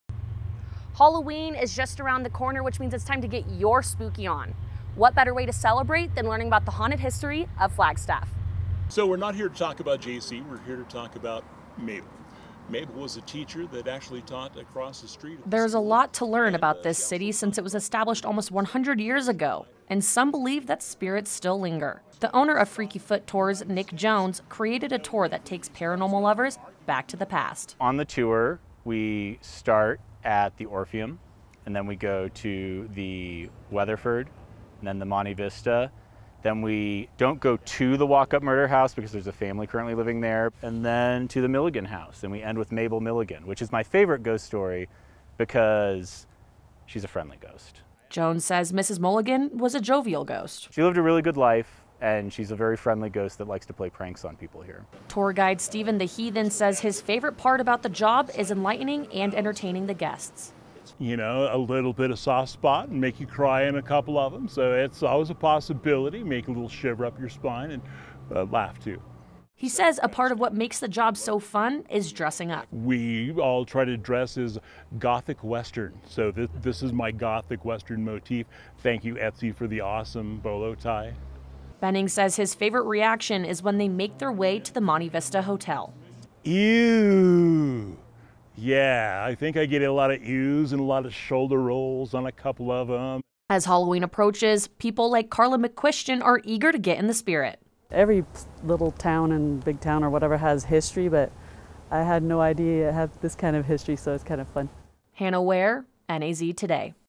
Freaky Foot Tours
NAZ Todays Hannah Ware spoke with Nick Jones, the owner of Freaky Foot Tours. The city of Flagstaff has been around for almost 100 years, which means it holds a lot of history. Some believe that spirits roam the streets of downtown to tell the tales.